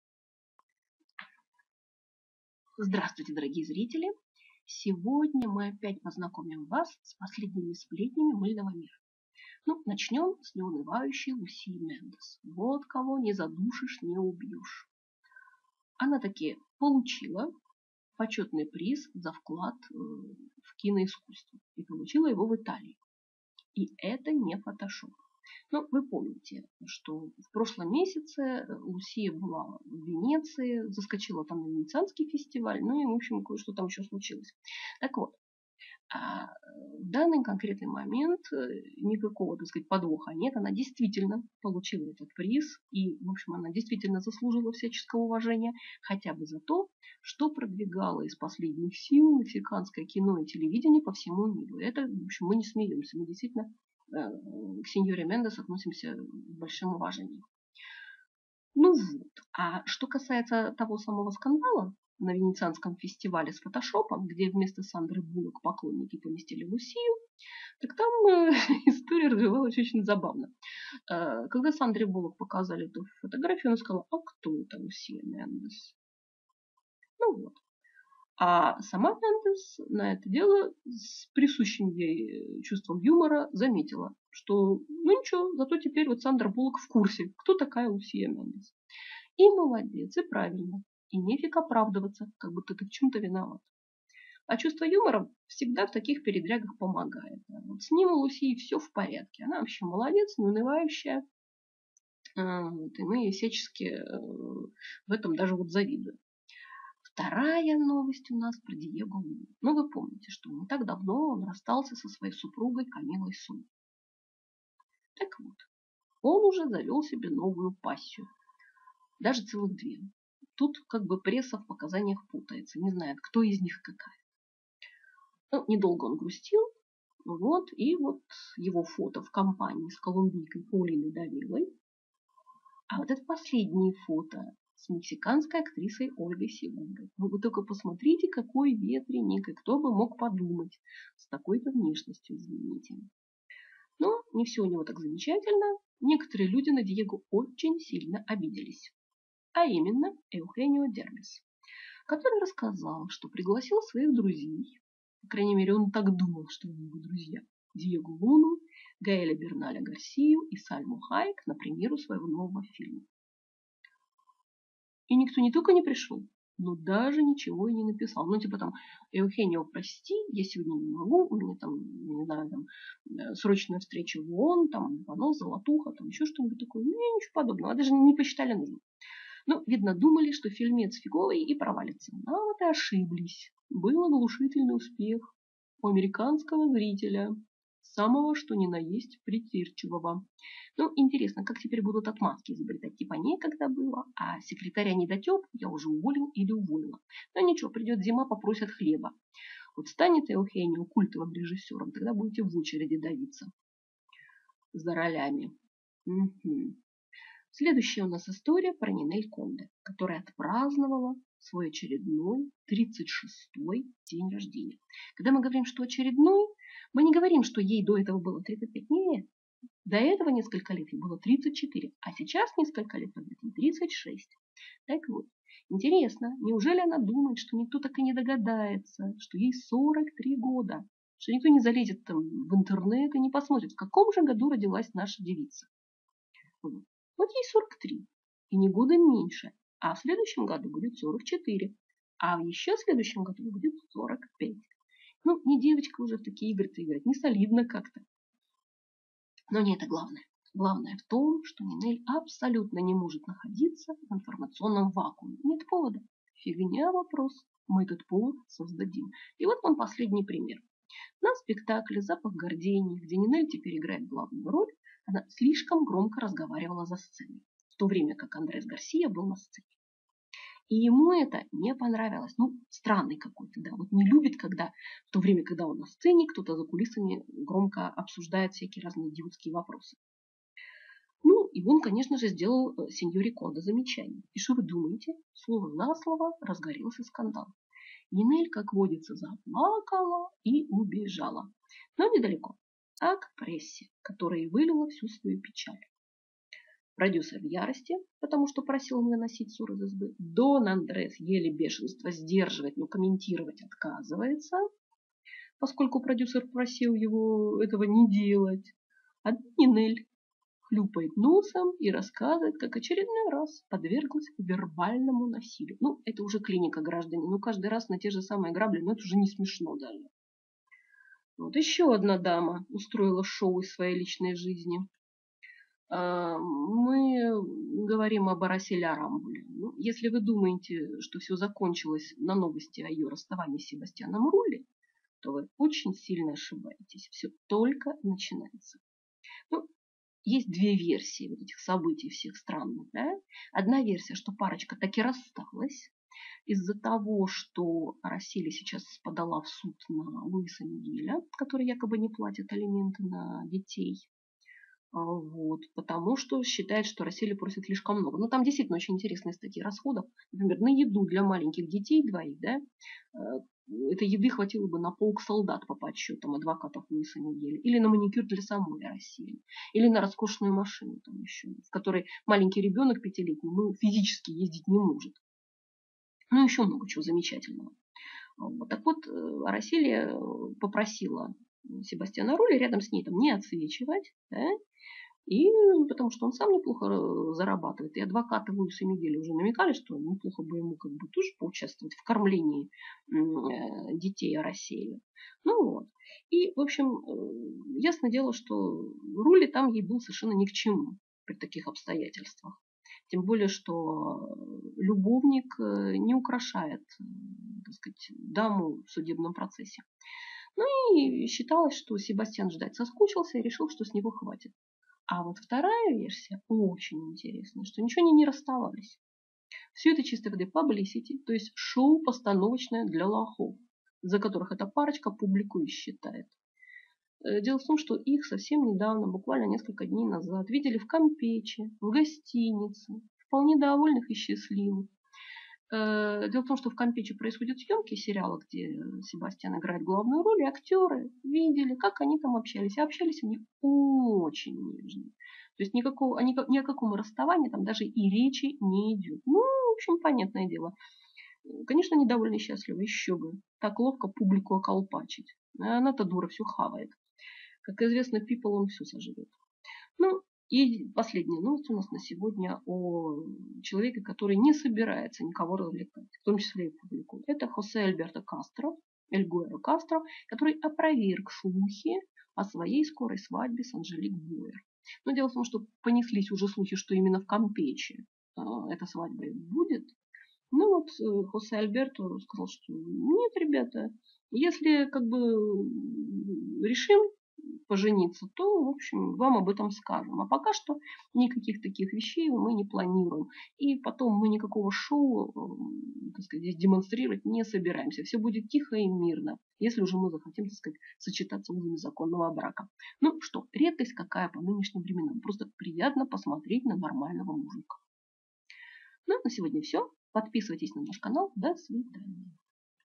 Soap Foam 70(October 2013)
Лусия Мендес получила почётный приз.Нинель Конде в своём репертуаре,устроила скандал с Андресом Гарсия.Диего Луна и его новые подруги.Эухенио Дербис обиделся на своих друзей.Новые повороты в истории Арасели Арамбулы и Себастьяна Рулли.Хосе Альберто Кастро отрицает скорую свадьбу.